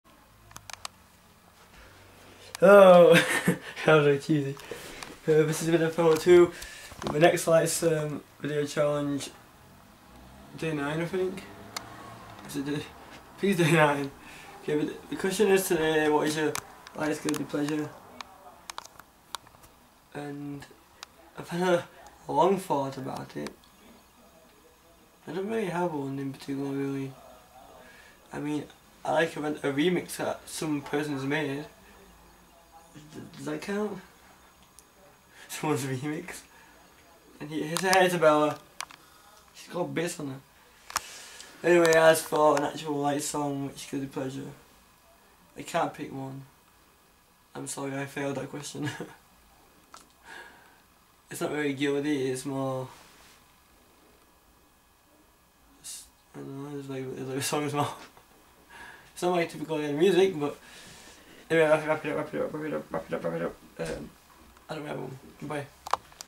31 Questions About Lights Question 9 (What Is Your Guilty Pleasure LIGHTS Song ?)
For March i am taking part on a 31 day challenge where i answer a question about lights each day, instead of just writing it i will make a video explaining things better anway this is question number 9.